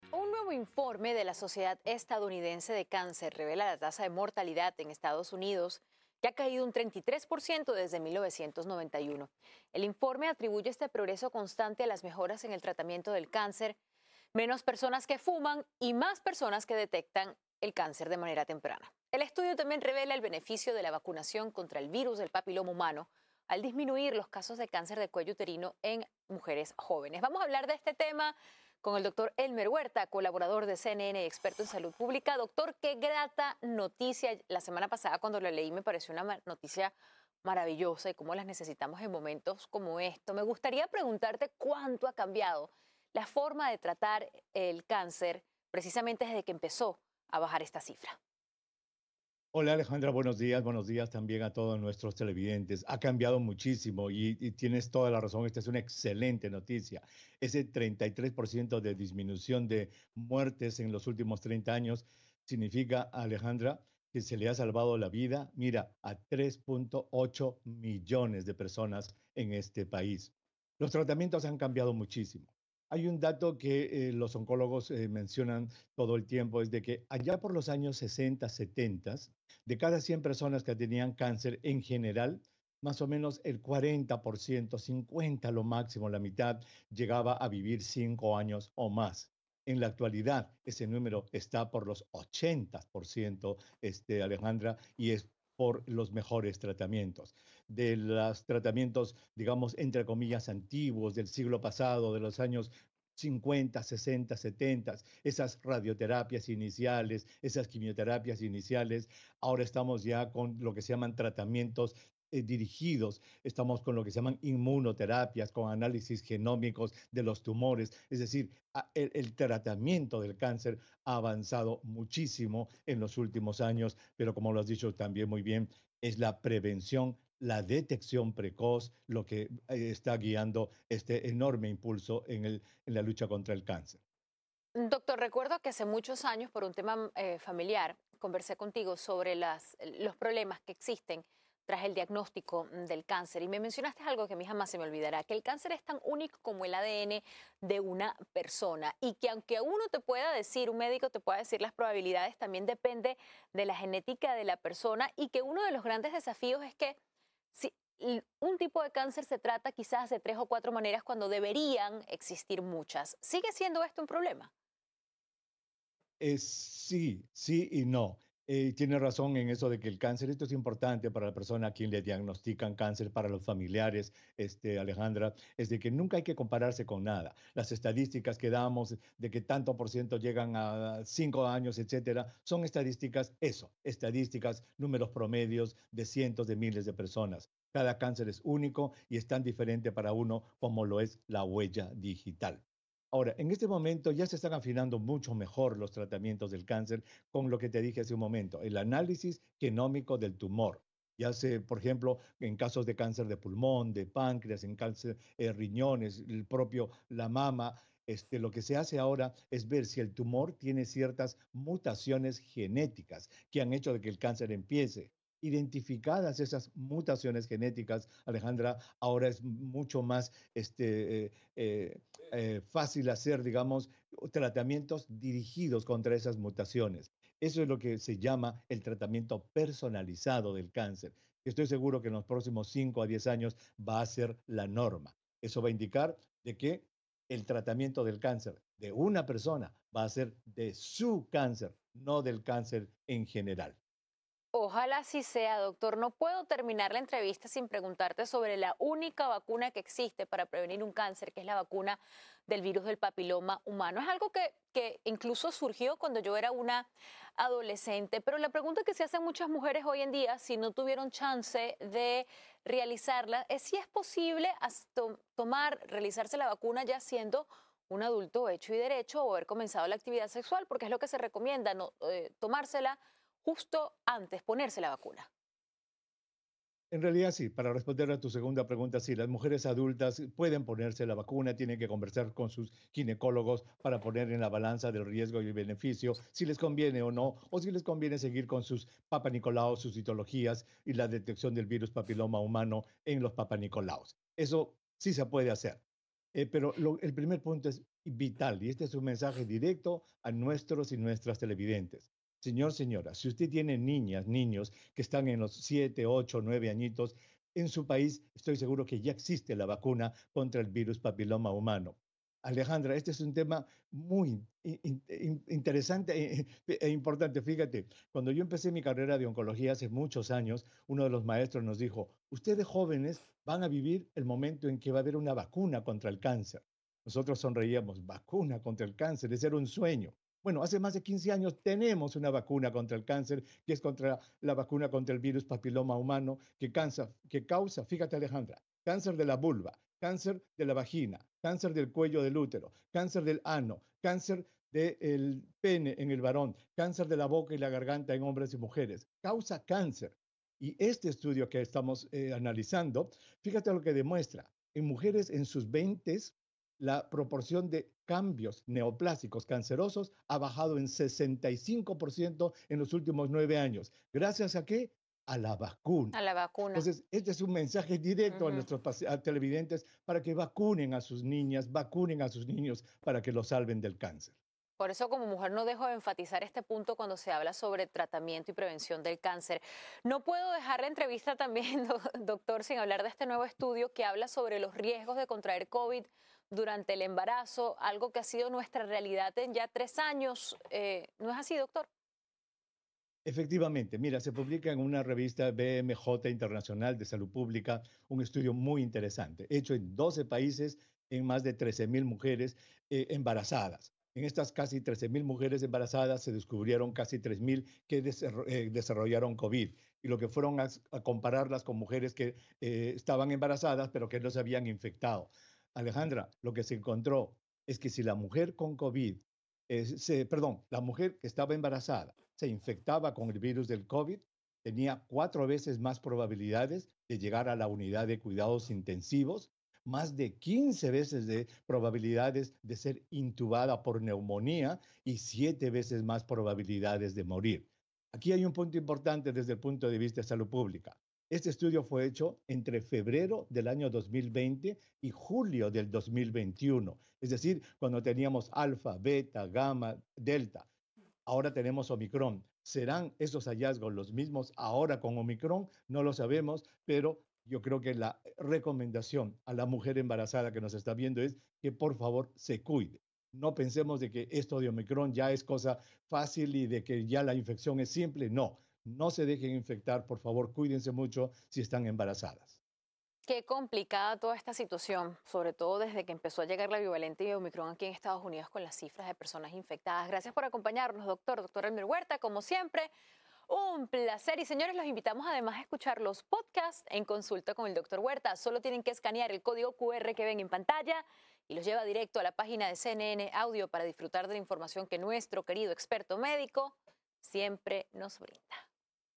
La tasa de mortalidad por cáncer en EE.UU. cae un 33%
Un nuevo informe de la sociedad estadounidense del cáncer revela la tasa de mortalidad en estados unidos ha caído un 33% desde 1991.
El informe atribuye este progreso constante a las mejoras en el tratamiento del cáncer, menos personas que fuman y más personas que detectan el cáncer temprano.